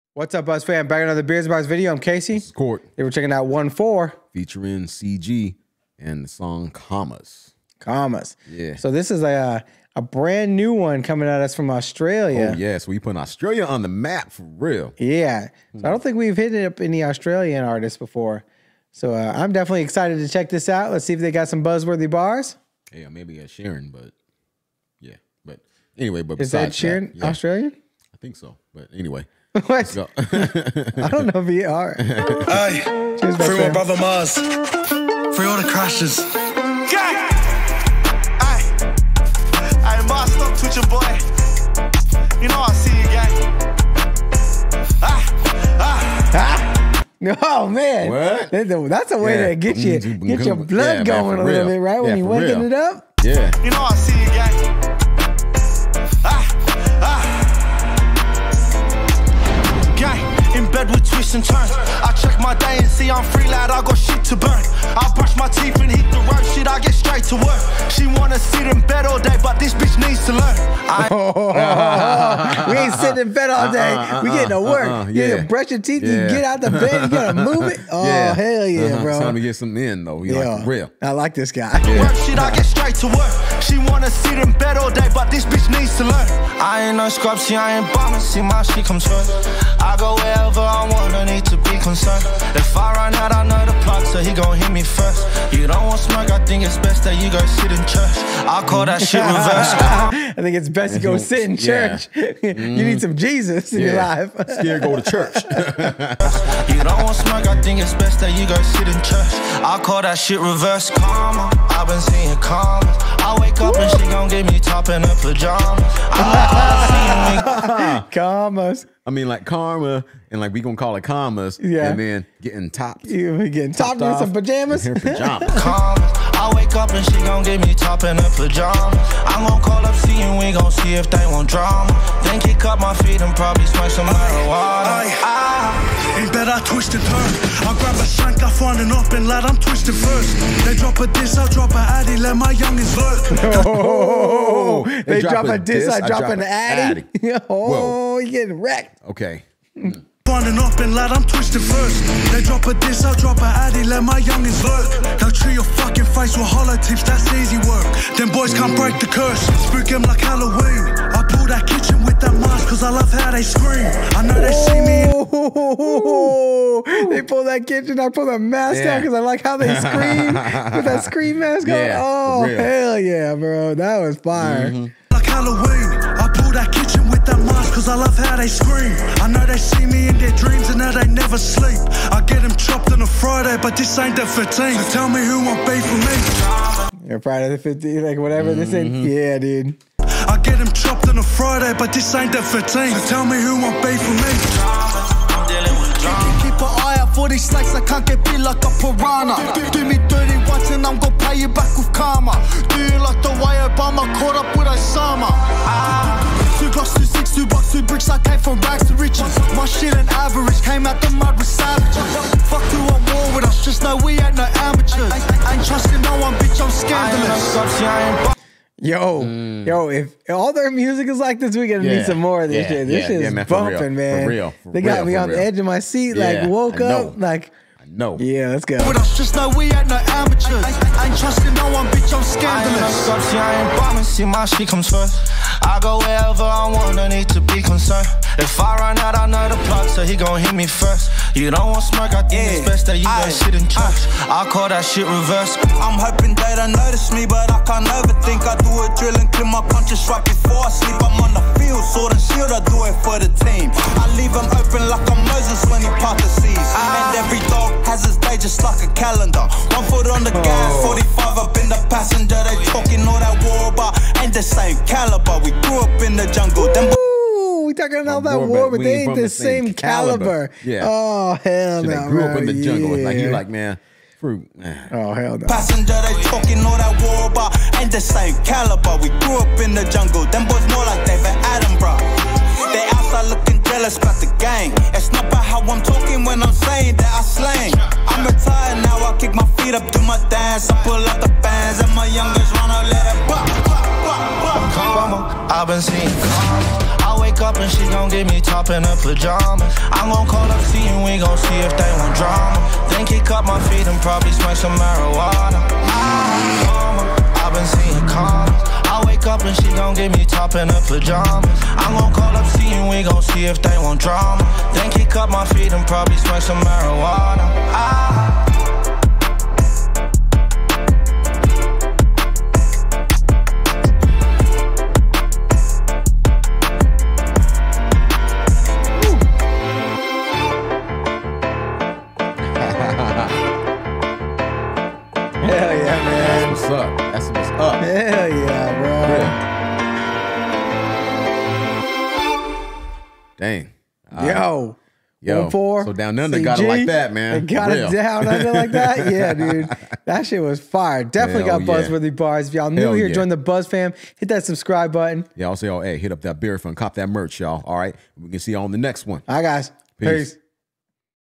AUSTRALIA GOT SOMETHING TO SAY!! | Americans React to Onefour ft. CG Comma’s
Welcome to our Onefour ft. CG Comma’s reaction!!! This is our latest video reaction from our new music reaction channel Beer’s & Bars. Here we listen, react, and breakdown bars while enjoying some of our favorite beers. Today as we extend our stay in the Down Under we were able to peep Onefour ft. CG Comma’s!